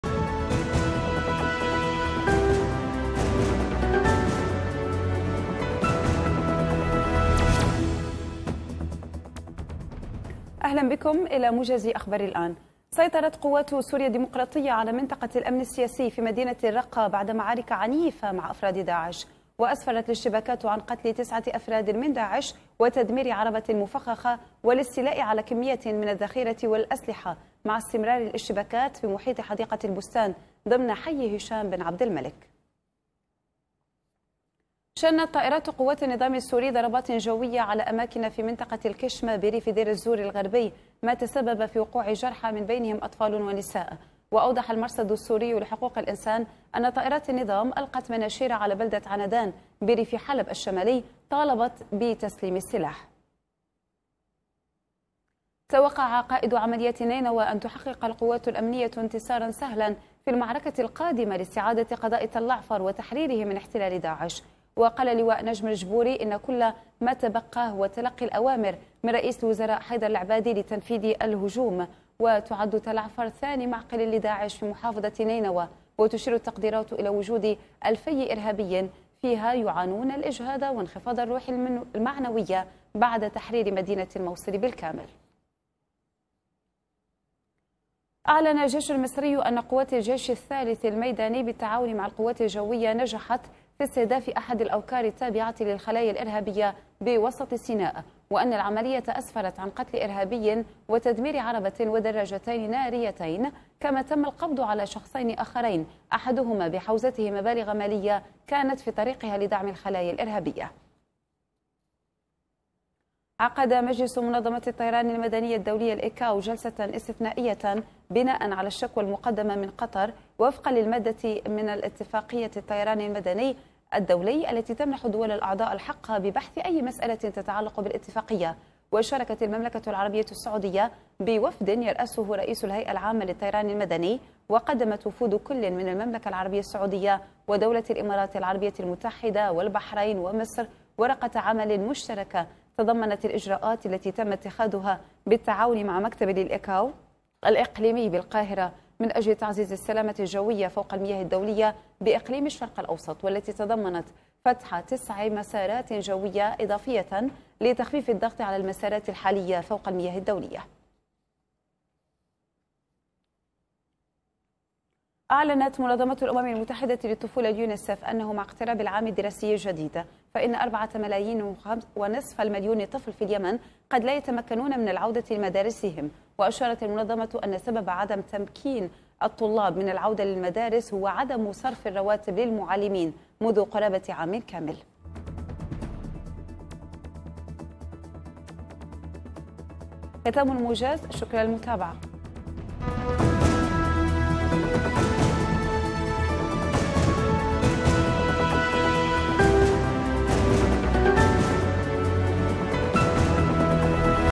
01-08-2017 | موجز الخامسة عصراً.. لأهم الأخبار من تلفزيون_الآن‎
العناوين:
- معارك عنيفة في مدينة
- نظام الأسد يقصف
- معركة تلعفر على الابواب
- الجيش المصري يداهم خلية إرهابية في
- الطيران الدولية ترفض طلب قطر بإدانة الدول المقاطعة
- اليونيسف:4.5 مليون طفل يمني لن يعودوا إلى مدارسهم